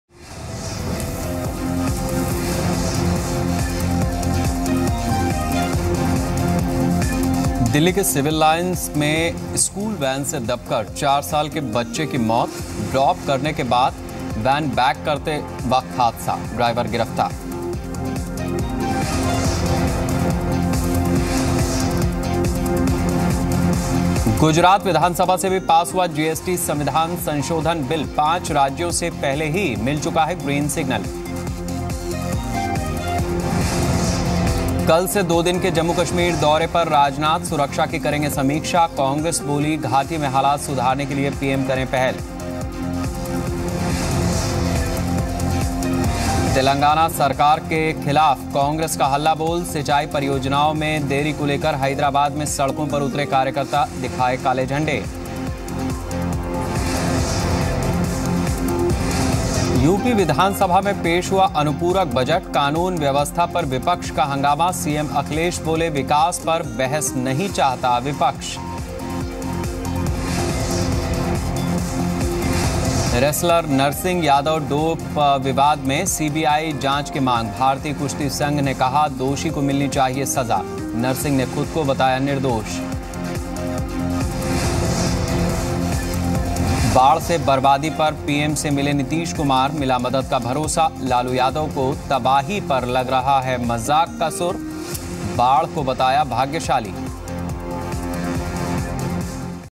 School Van kills a 4-year old school boy in Delhi's Civil Lines area
School Van kills a 4-year old school boy in Delhi's Civil Lines area Watch complete news story of Headlines @ 7:00 PM for getting detailed updates!